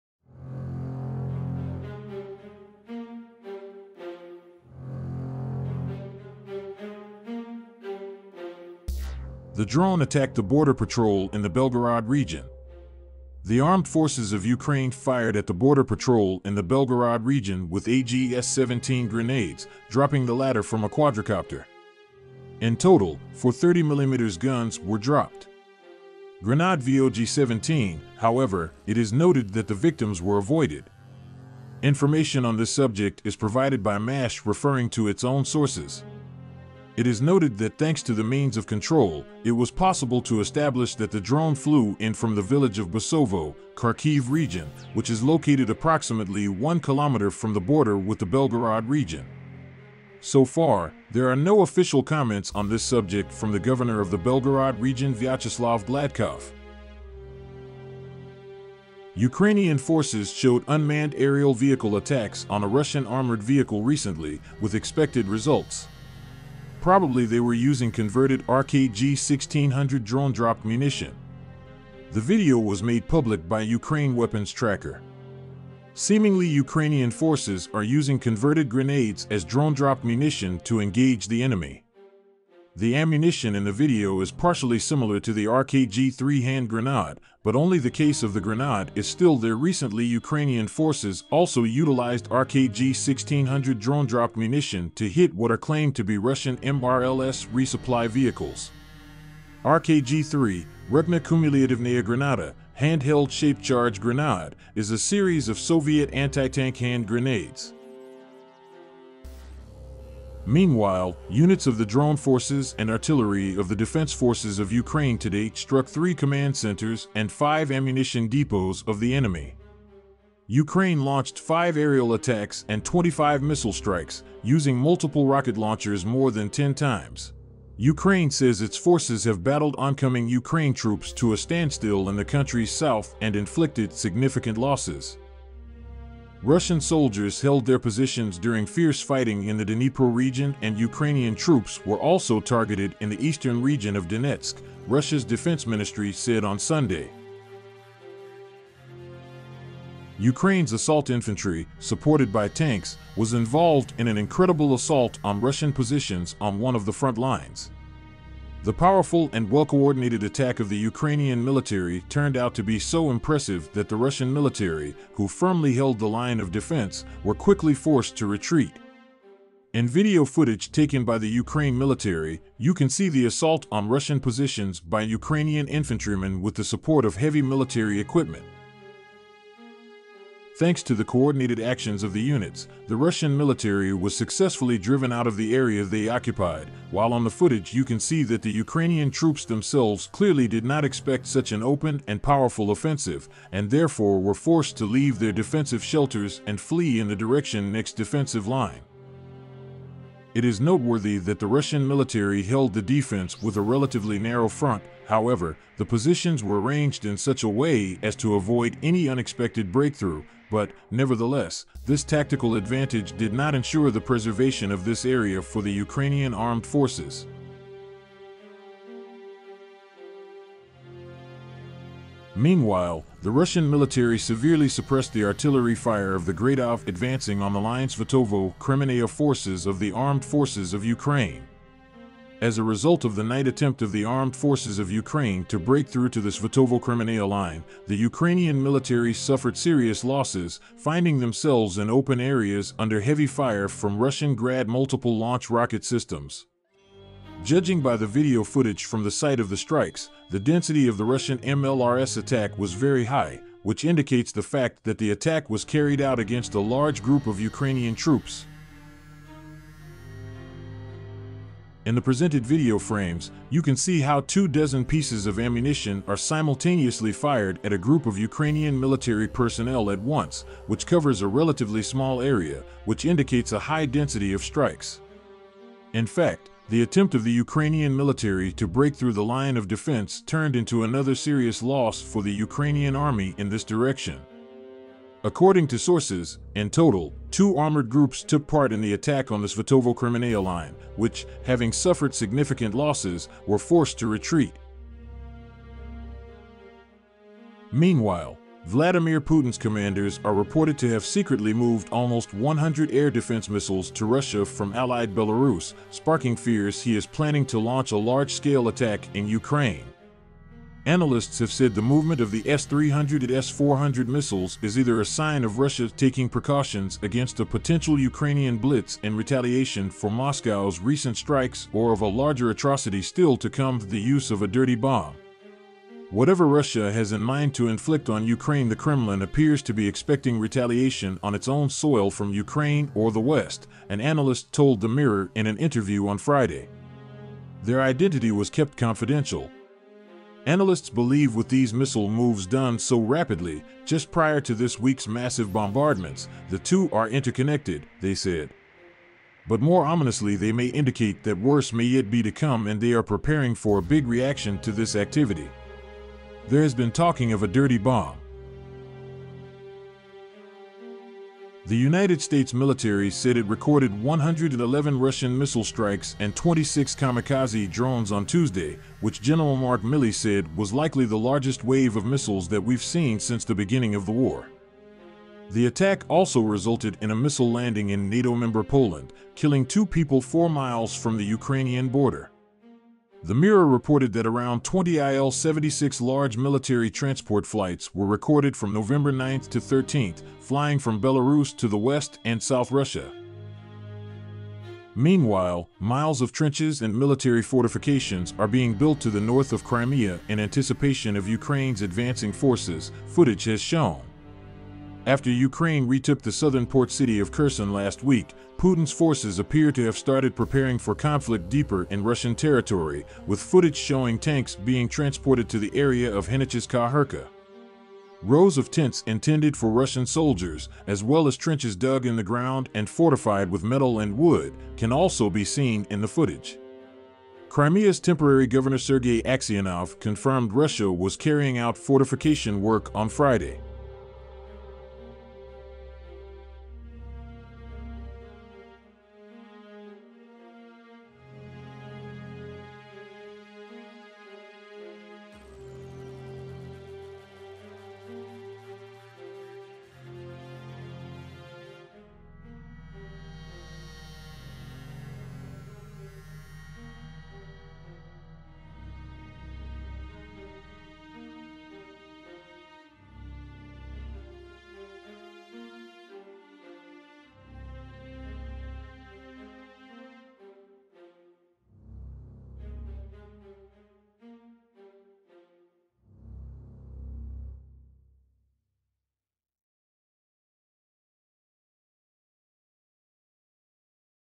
Victory footage! Ukraine Drones Dropping Bombs that Destroy Russian tanks and troops on front line
Ukraine Drones Dropping Bombs that Destroy Russian tanks and troops on front line. Ukrainian forces showed unmanned aerial vehicle attacks on Russian tanks recently, with expected results. Ukrainian Armed Forces drop Modified Drone Grenade on Russian tank patrolling the front line